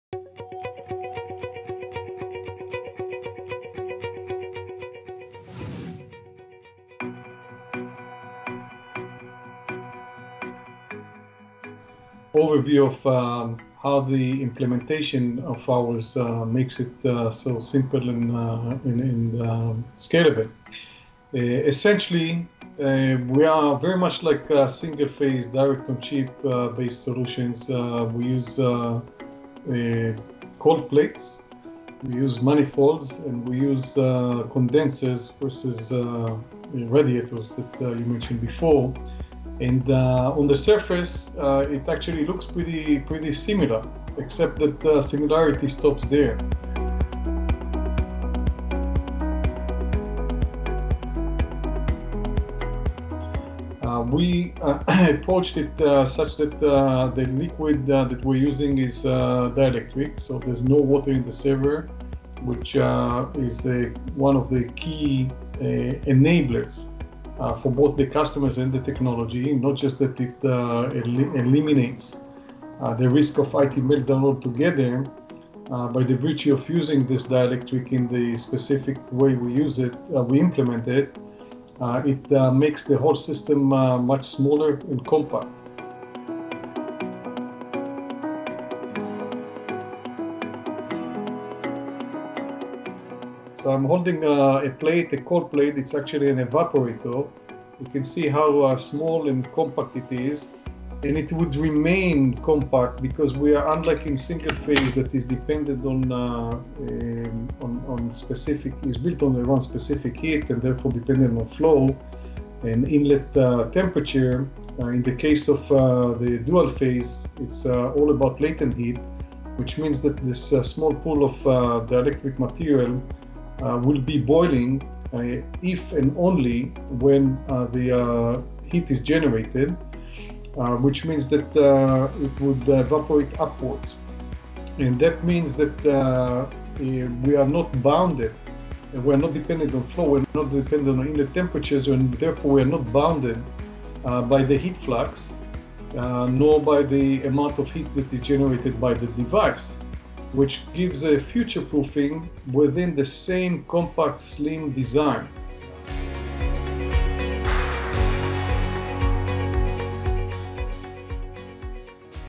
Liquid Cooling for the Mainstream: Overview of the ZutaCore HyperCool solution
@StorageReview and Udi Paret, President US of ZutaCore discuss the next-generation dielectric liquid cooling solution that makes cooling simple and scalable for data center owners and operators.